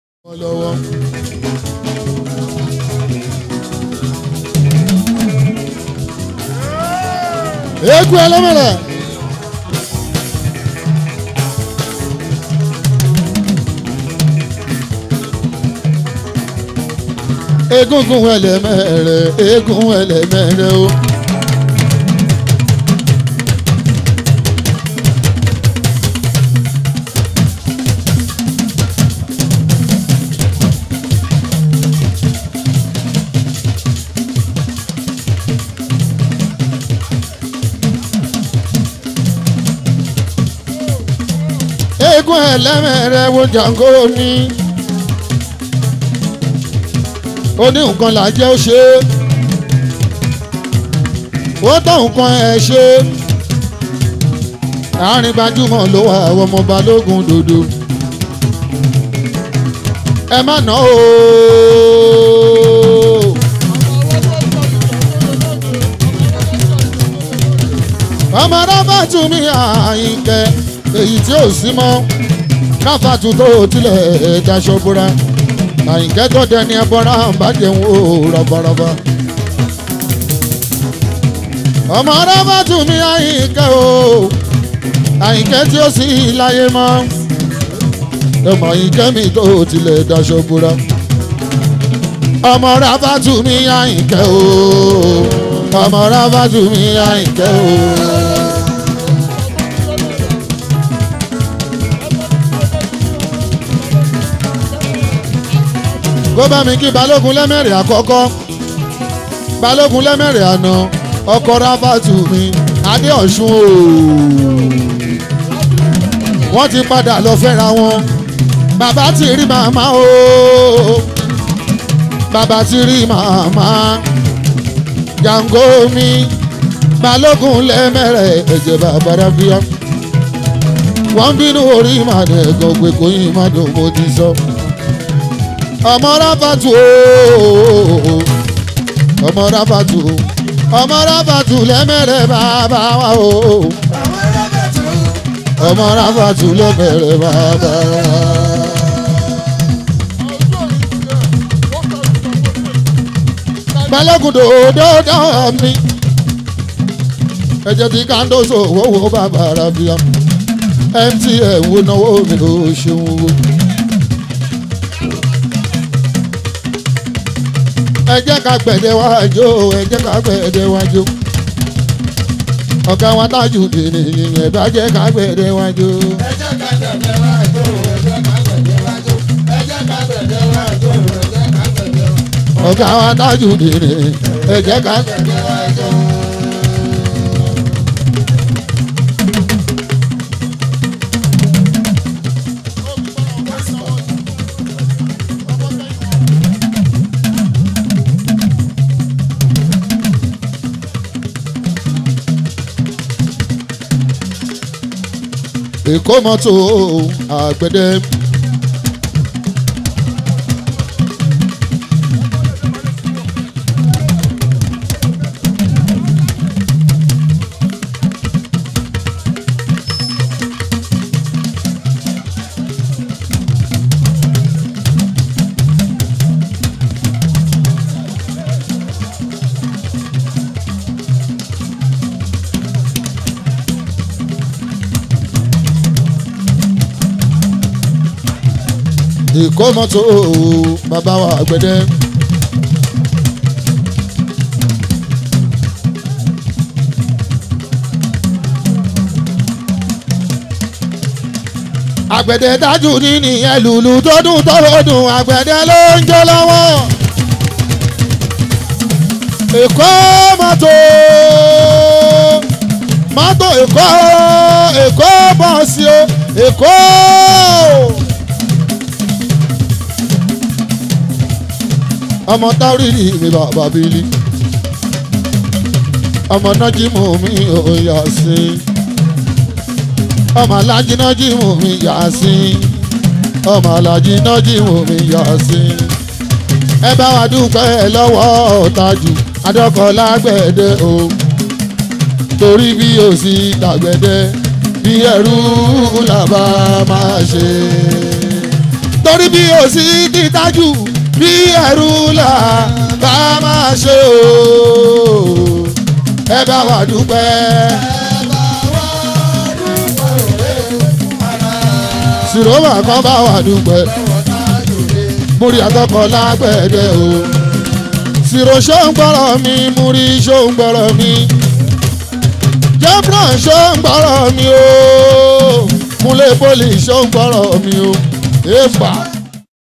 Alh Shefiu Alao live performance
Alhaji Shefiu Alao live performance at ALH JANGO'S mother burial party